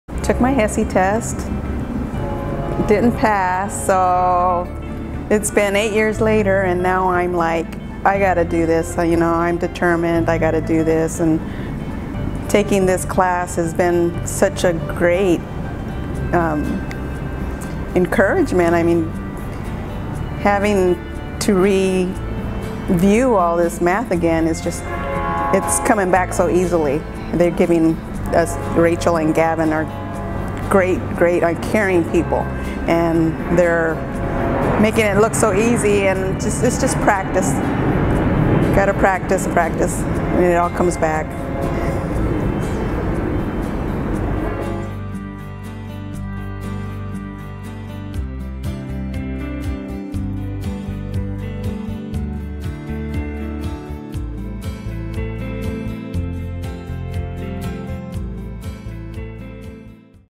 Power Math Camp: Sandra's Story
Sandra participated in the FREE Power Math Camp offered in August 2017, and was willing to share what she thought of the experience!  Learn how Power Math Camp helped!

Power Math Camp is a free opportunity to review your essential math skills.  or join today!